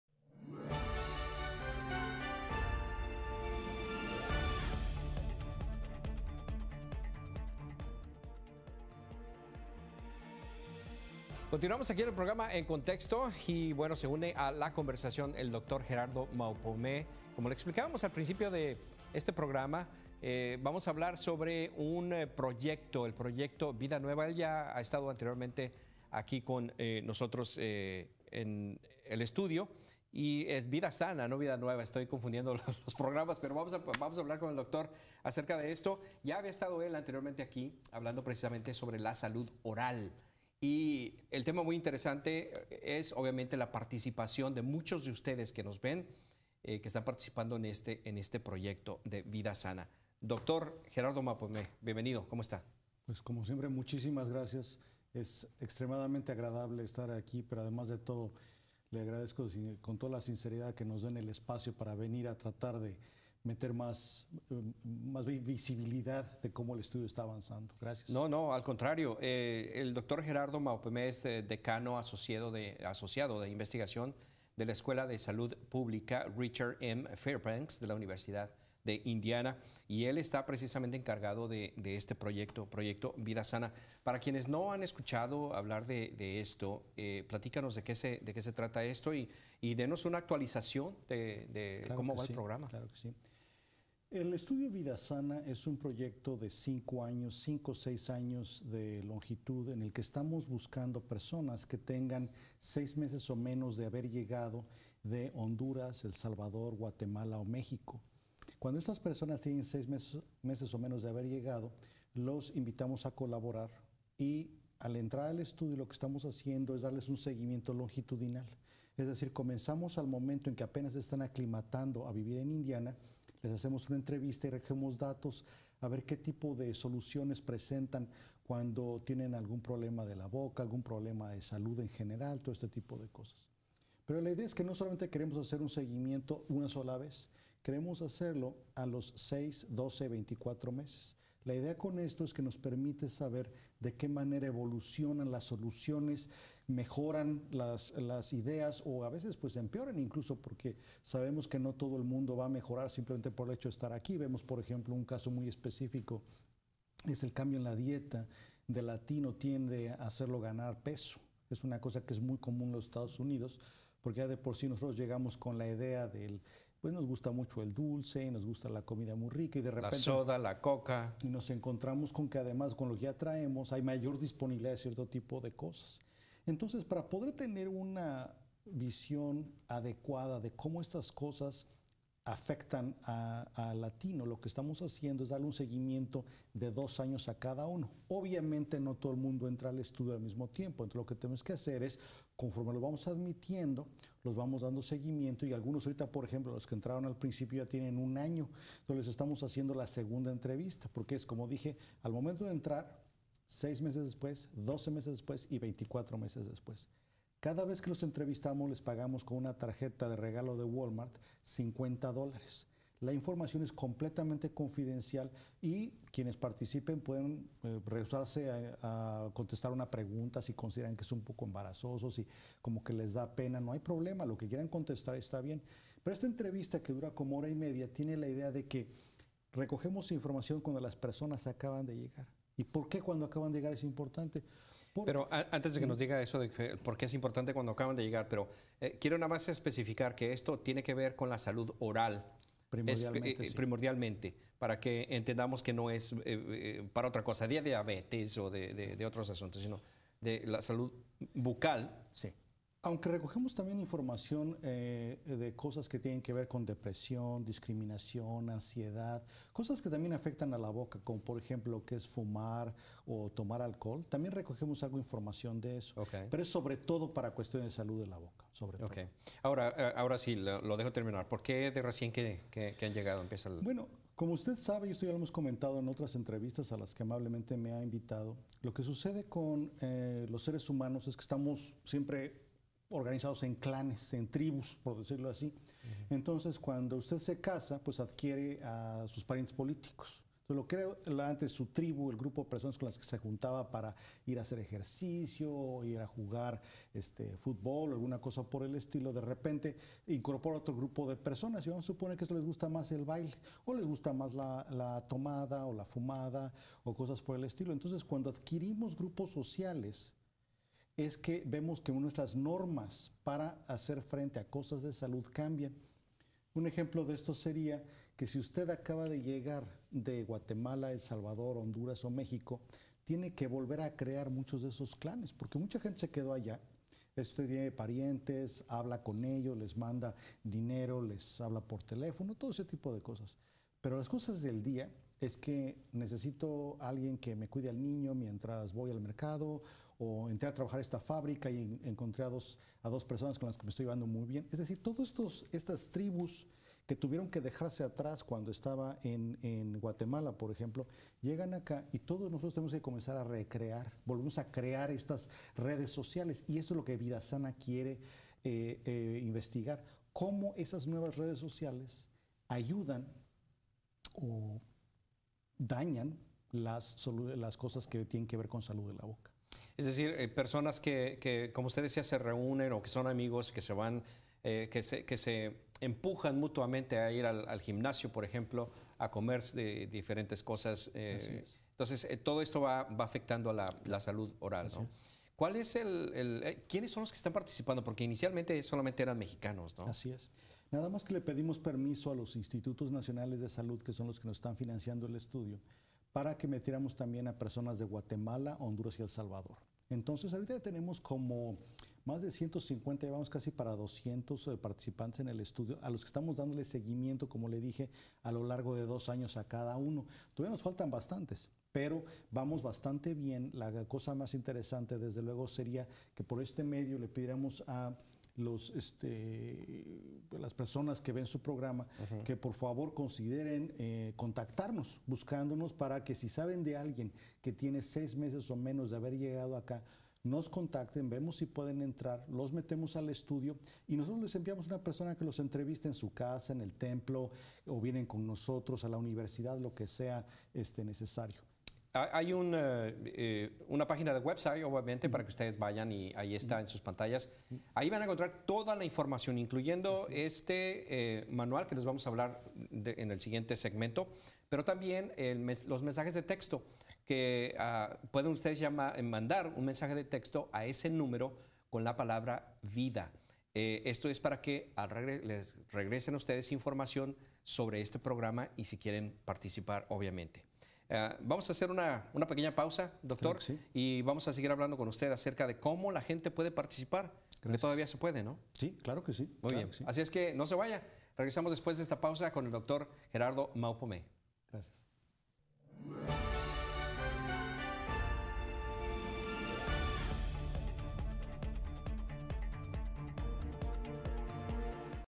Hispanos en Indiana participan en Proyecto Vida Sana - Dr. Gerardo Maupomé. PARTE 1
El Dr. Gerardo Maupomé, investigador de Salud Oral de la Universidad de Indiana habla con Luis Navarro sobre el avance del proyecto Vida Sana, en la que participan hispanos en Indiana.